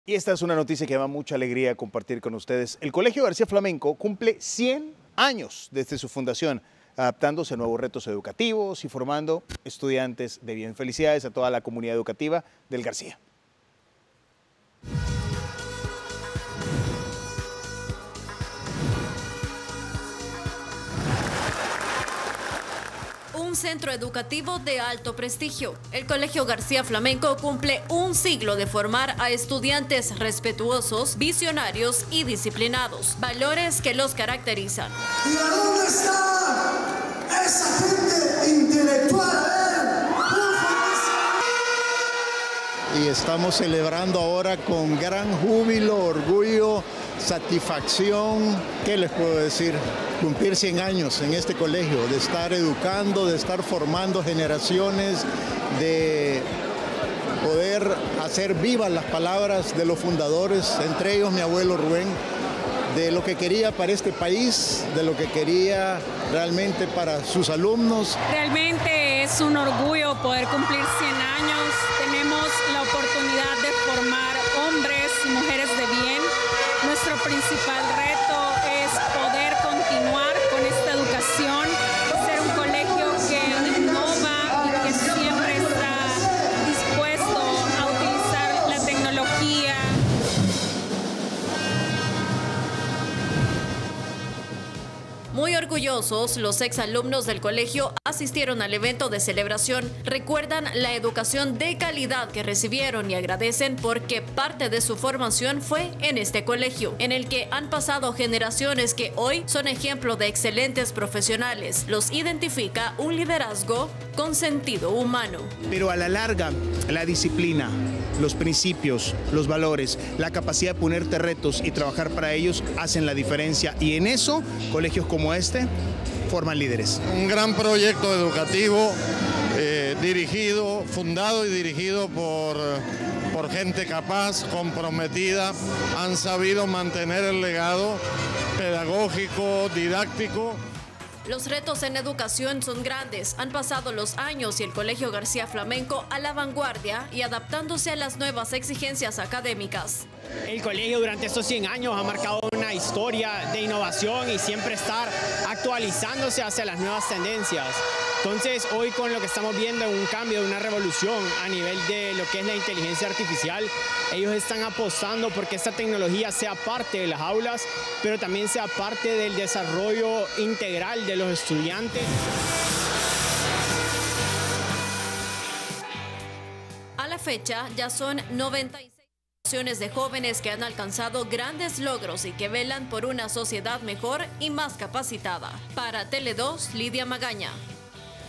Evento Colegio García Flamenco
El Colegio García Flamenco cumple 100 años desde su fundación, adaptándose a nuevos retos educativos y formando a estudiantes de bien.